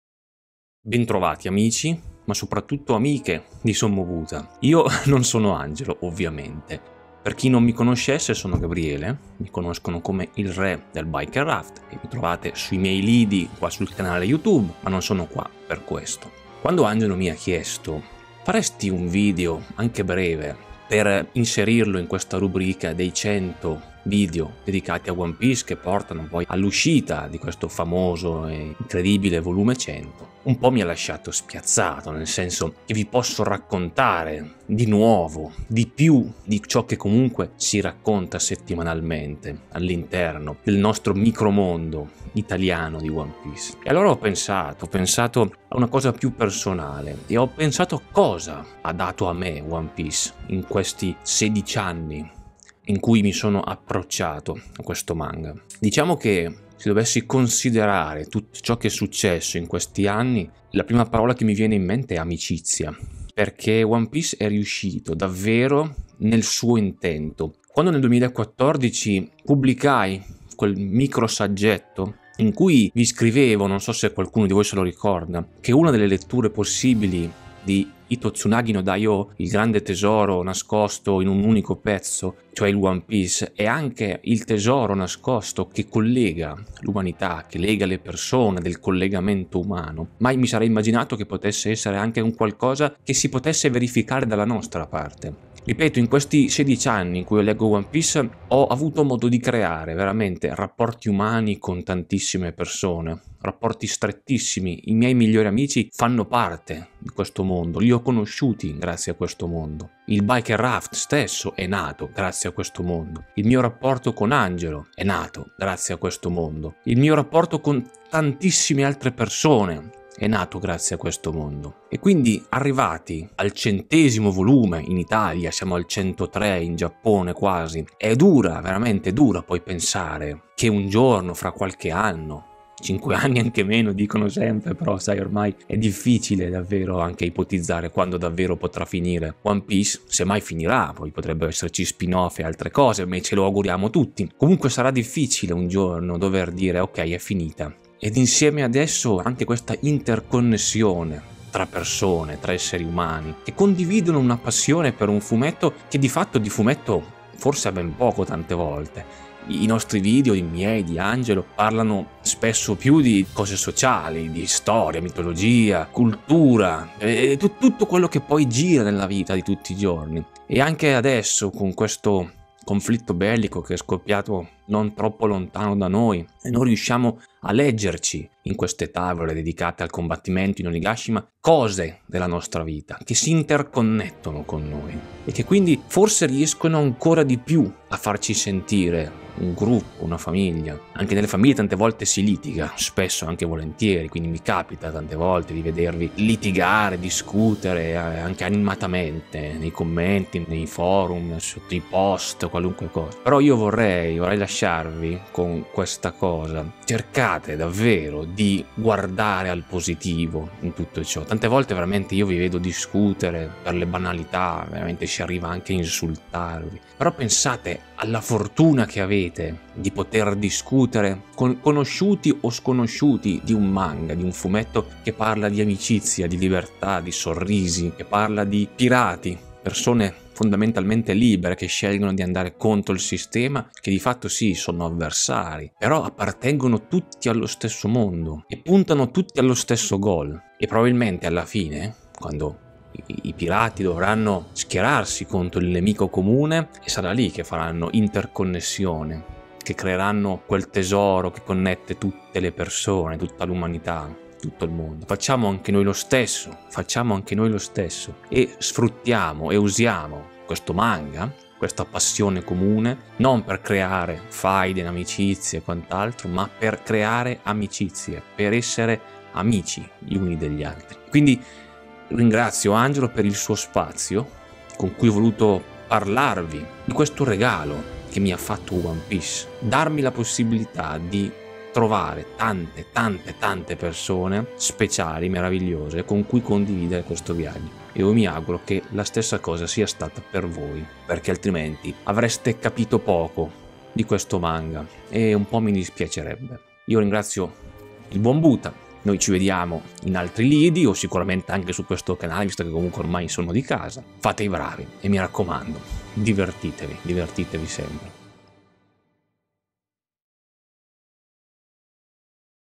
COSA CI HA REGALATO ONE PIECE IN QUESTI ANNI? | 100 cose su ONE PIECE 0035
Una riflessione su ONE PIECE
▼▼CONTINUA SOTTO▼▼
✪ I MIEI LIBRI ✪
LEGGILI QUI ➜